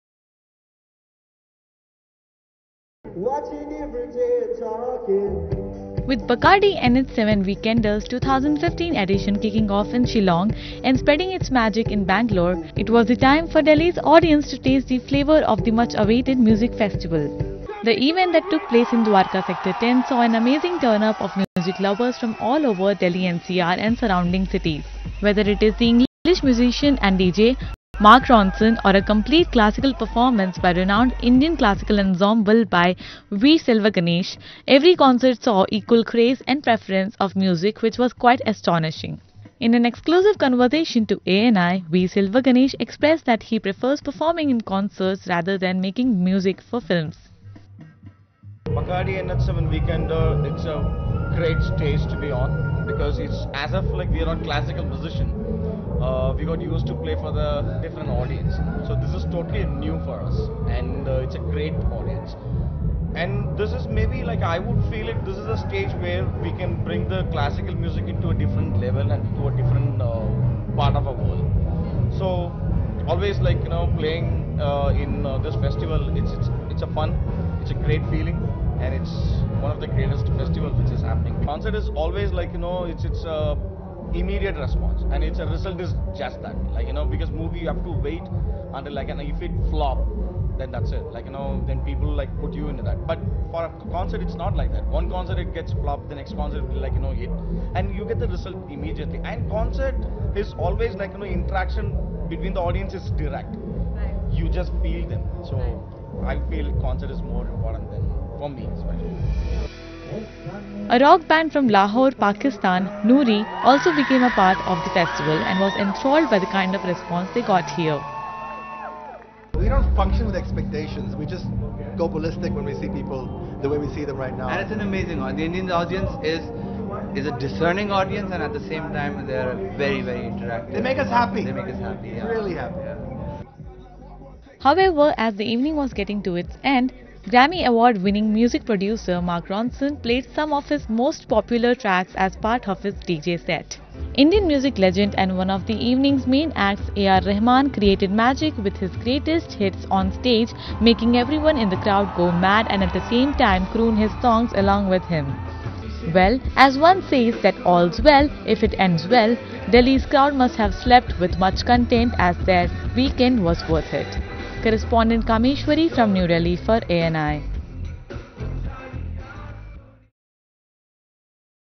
Delhi tastes every music genre at BACARDI NH7 Weekender's Day 1
New Delhi, Nov 29 (ANI): With BACARDI NH7 Weekender's 2015 edition kicking off in Shillong and spreading its magic in Bangalore, it was the time for Delhi's audience to taste the flavour of the much awaited music festival. The event that took place in Dwarka sector-10 saw an amazing turn up of music lovers from all over Delhi NCR and surrounding cities. Whether it is the English musician and DJ Mark Ronson or a complete classical performance by renowned Indian classical ensemble by V. Selva Ganesh, every concert saw equal craze and preference of music which was quite astonishing.However, as the evening was getting to its end, Grammy Award-winning music producer Mark Ronson played some of his most popular tracks as part of his DJ set. Indian music legend and one of the evening's main acts A. R. Rahman created magic with his greatest hits on stage making everyone in the crowd go mad and at the same time, croon his songs along with him.Well, as one says that all's well if it ends well, Delhi's crowd must have slept with much content as their weekend was worth it.